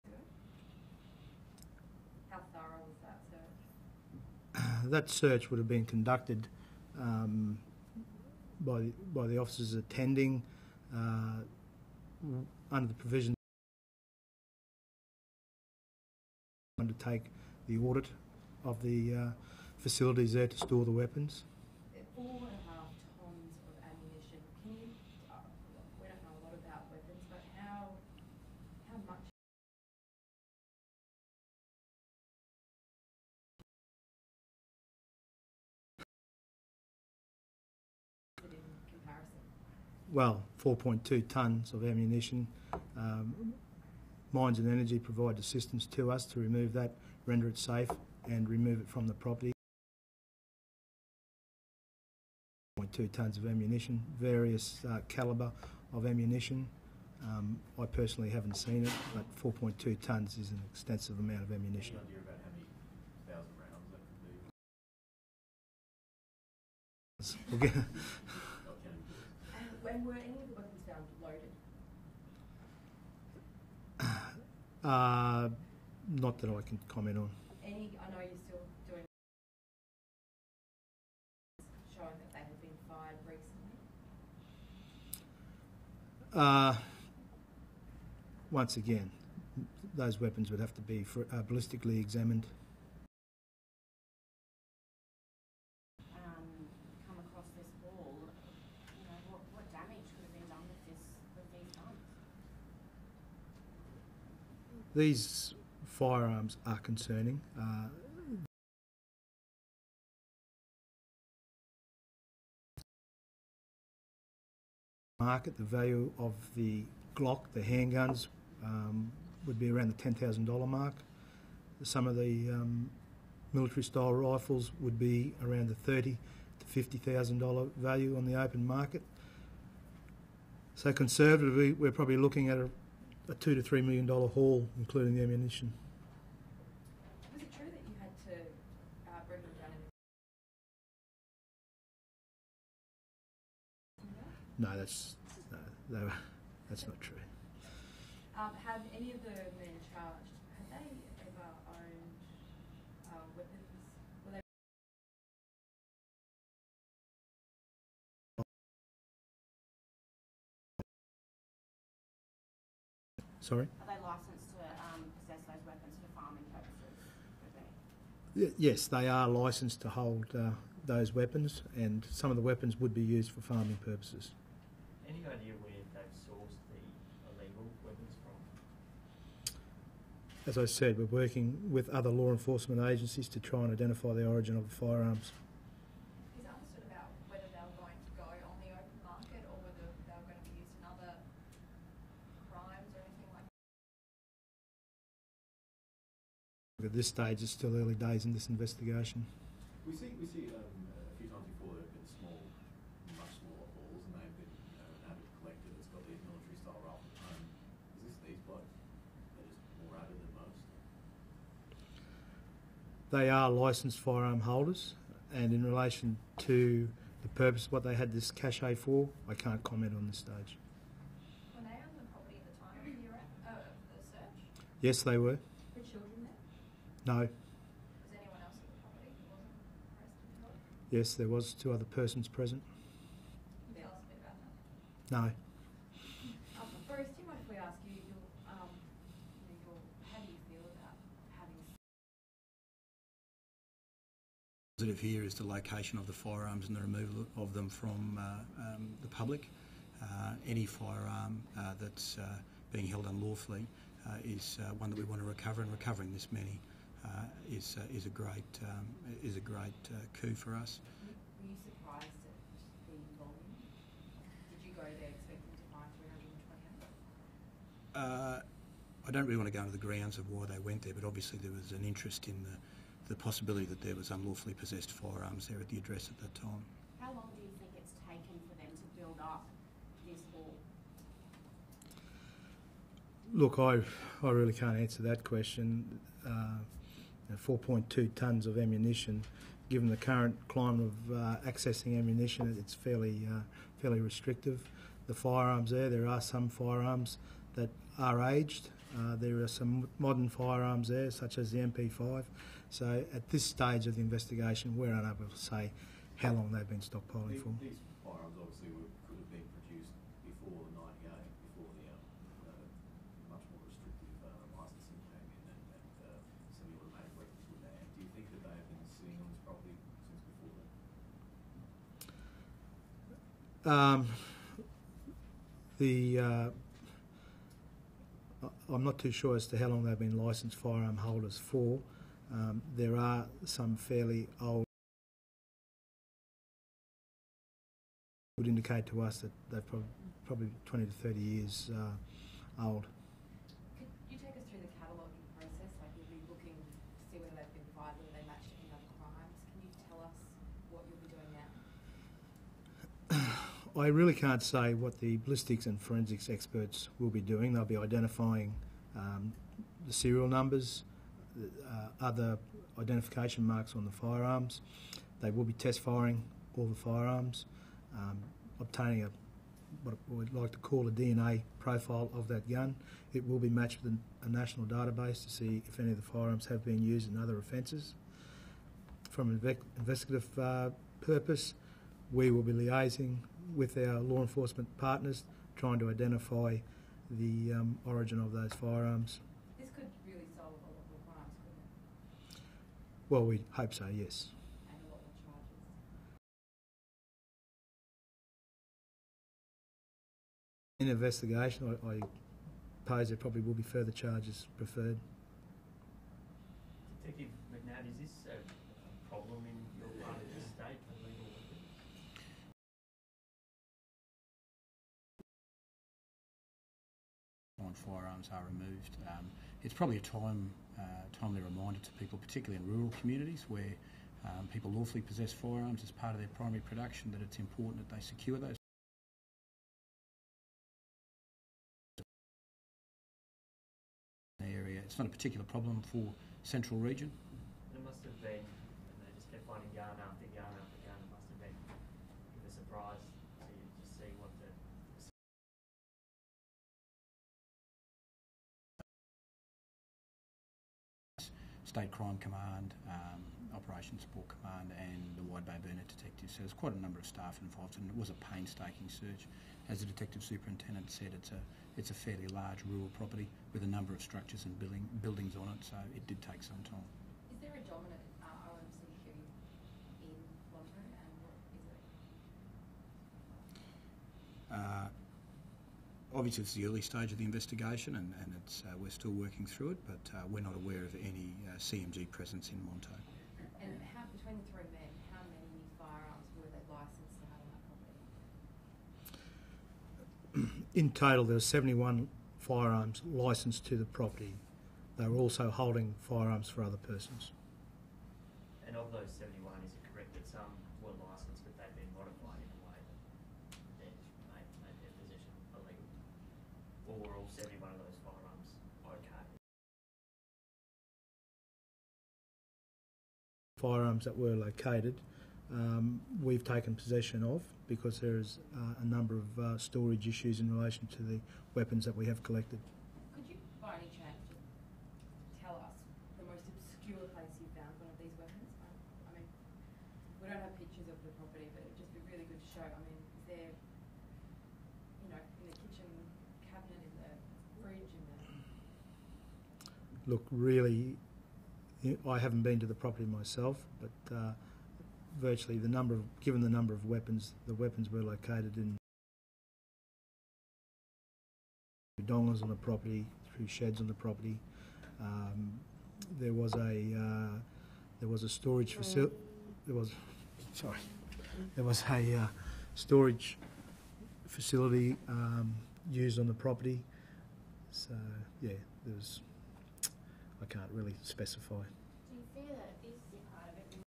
Media Conference - Weapons Seized from Monto
Media Conference following the location of one of the largest holdings of illegal firearms, military style weapons, and ammunition in Queensland following an extended search of a property in Monto.